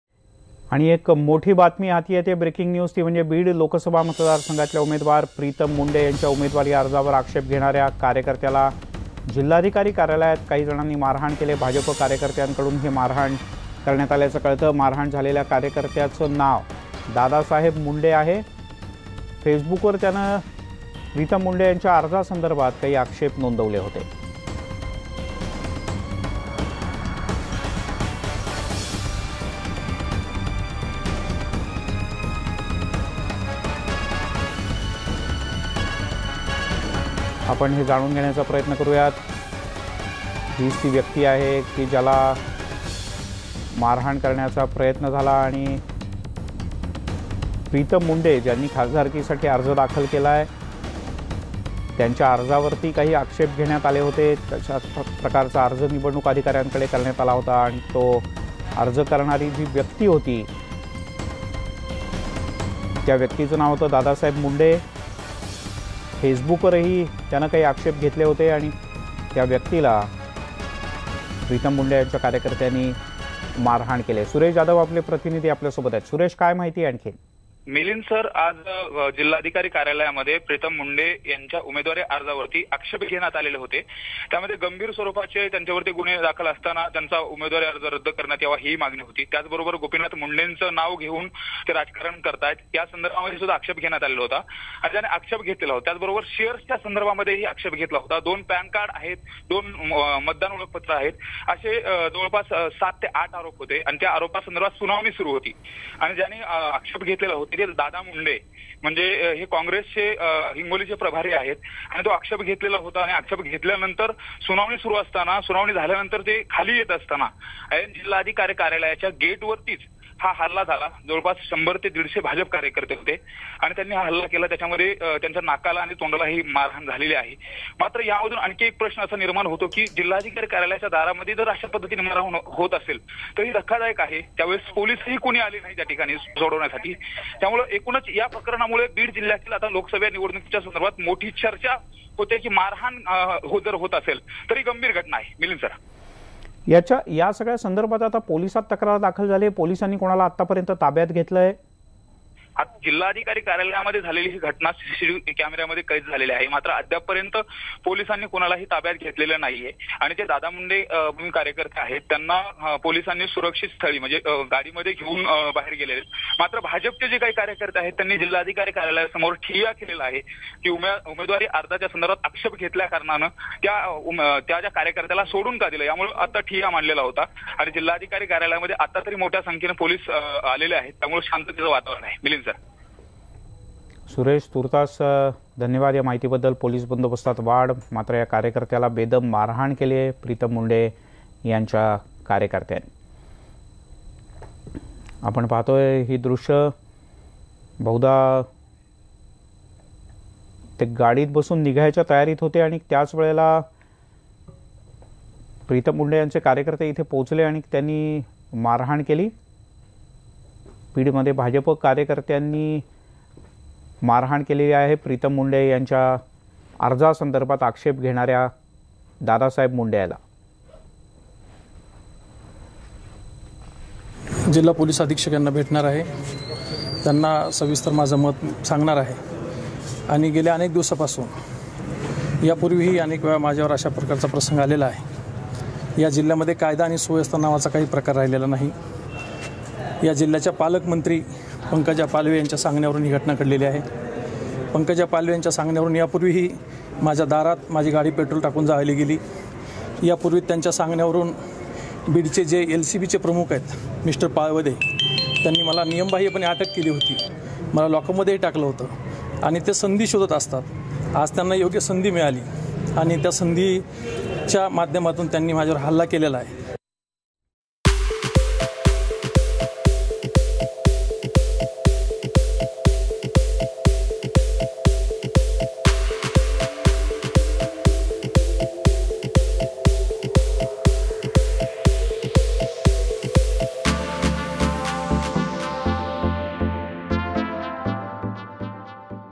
Election2019: प्रीतम मुंडेंच्या अर्जावर आक्षेप घेणाऱ्याला बेदम मारहाण | News18 Lokmat
Election2019: प्रीतम मुंडेंच्या अर्जावर आक्षेप घेणाऱ्याला बेदम मारहाण |दादासाहेब मुंडेंनी फेसबुकवरून अर्जावर घेतला आक्षेप